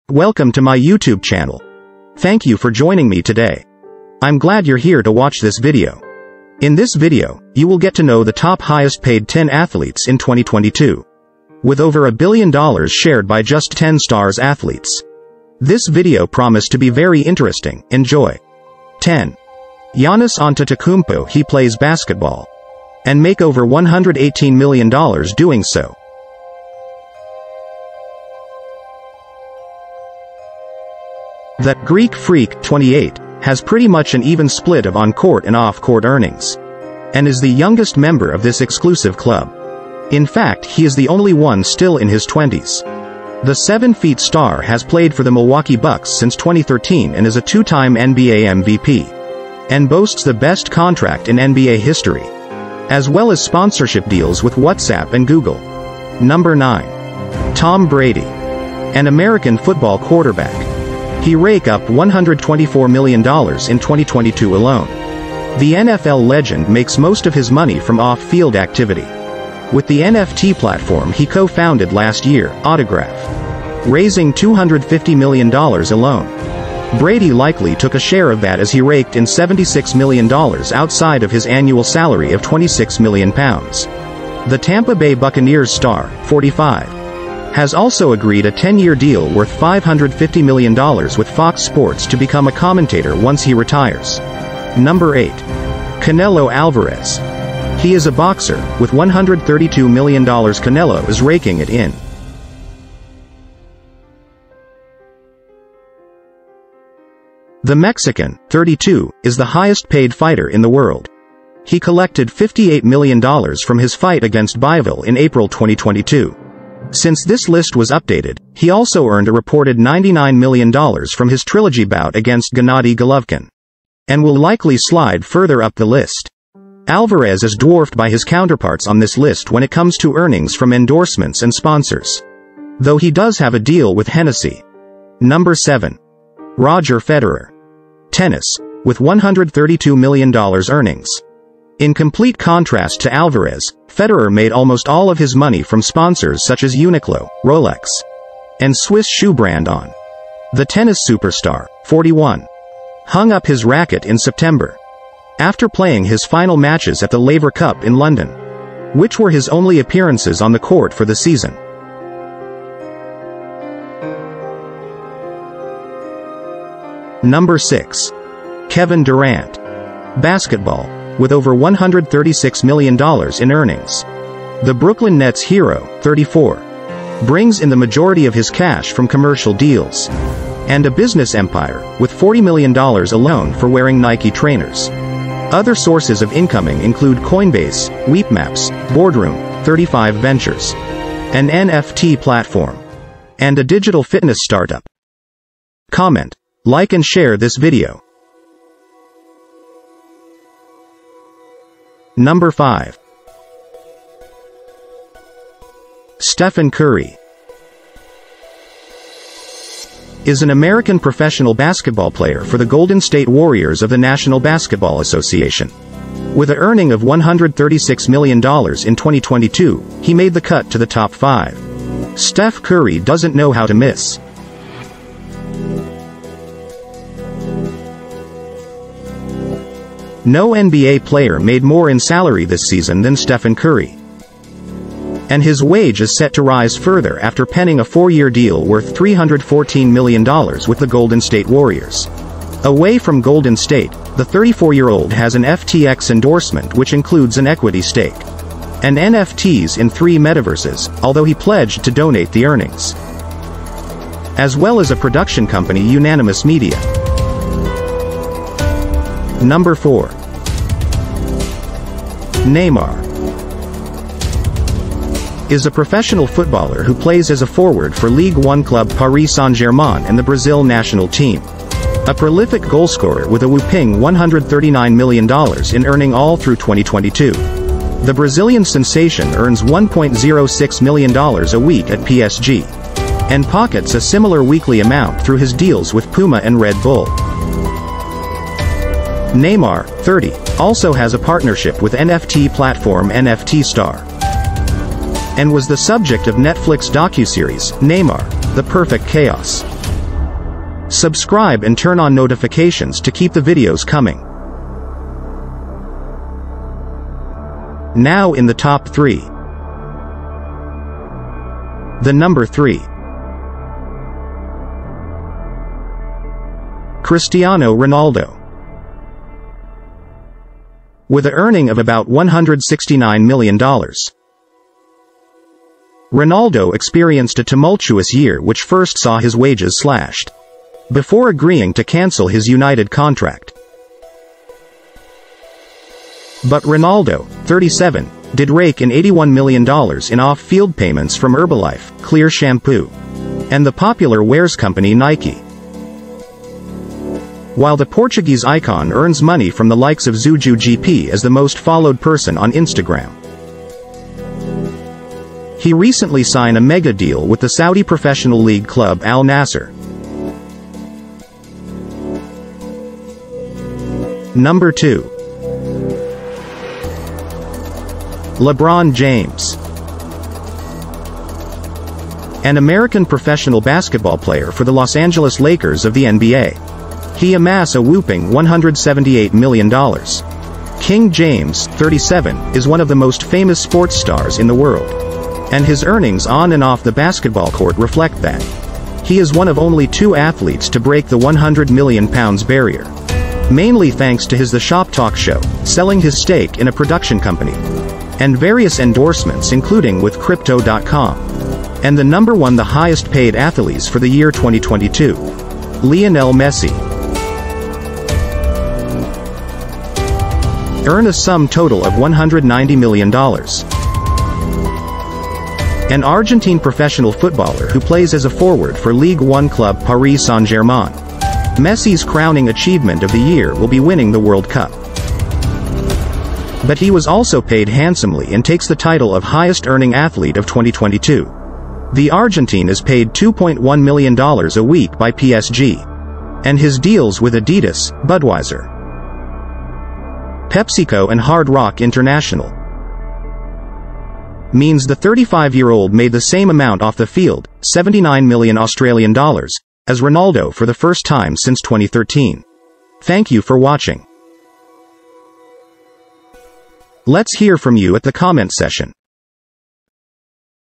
2022 Top 10 Highest-Paid Athletes in Sports: Where They Rank
2022 Top 10 Highest-Paid Athletes in Sports: Where They Rank

Welcome to Luxury Splendour

2022 had professional athletes back in the limelight, doing what they do best. The highest-paid athletes showcase the best in human capability, however, it was what these athletes earned off the field that really made headlines in this year’s Forbes highest-paid athletes list.

Footballer and PSG star Lionel Messi took home the title of this year’s highest-paid athlete, earning a massive payday in millions of dollars for 2022, with over $1 Billion share among them

In this video, you will get to know who and what position your favorite athlete rank

 million of it coming from endorsements and brand deals off the field.

The Forbes official list of the highest-paid athletes is upon us. And this year, we’ve got a list full of huge paydays for athletes both on and off the playing field.

2022 had professional athletes back in the limelight, doing what they do best.

The highest-paid athletes showcase the best in human capability, however, it was what these athletes earned off the field that really made headlines in this year’s Forbes highest-paid athletes list. Footballer and PSG star Lionel Messi took home the title of this year’s highest-paid athlete, earning a massive $130 million payday for 2022, with $55 million of it coming from endorsements and brand deals off the field.

The 10 highest-paid athletes in the world took home total pretax earnings of $992 million during the last year. And as the Forbes graph below shows, that’s a 6% decrease on last year’s earners that were led by Connor McGregor ($180 Million) who doesn’t even make the list this year. Last year marked the first time four athletes earned over $100 million, however, this year we only find two – two football legends and a basketballer.

2018 saw the largest collective total in Forbes history, and this year saw the total pretax earnings rank third all time. While 2021 saw the largest off-field total earnings of all time in the wake of a global pandemic, this year fell short by only a few million proving how athletes are as savvy and marketable as they are skilled. Now, where’s that list?

Forbes top ten
Image: Forbes

A global pandemic wasn’t enough to stifle these competitors in 2021, with a notable increase in off-the-field sponsorship deals across the board and the same can be said for 2022. Only two athletes managed to eclipse the $70 Million mark in off-field earnings this year, Lebron James and Rodger Federer. Who else made it large?

Without further ado, here’s the official list of the 10 Highest-Paid Athletes for 2022.